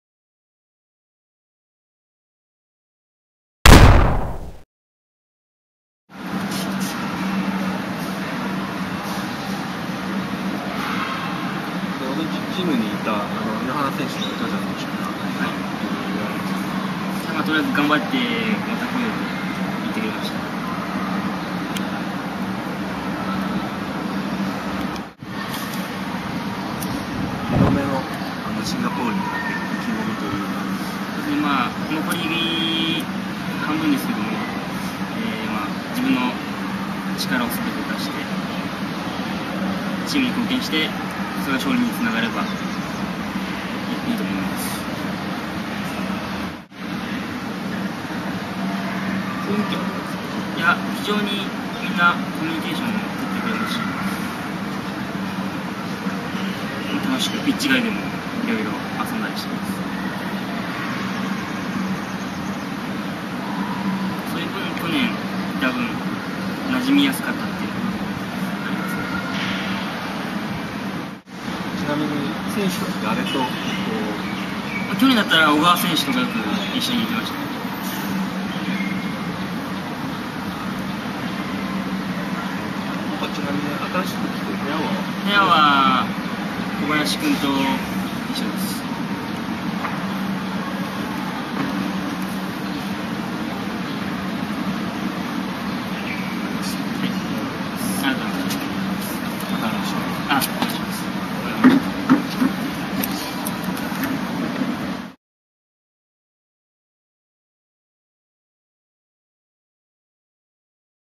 取材（小森選手編）②
2008.7.7に行われた週刊Jスポの取材。
小森由貴選手が取材を受けている様子をご覧下さい。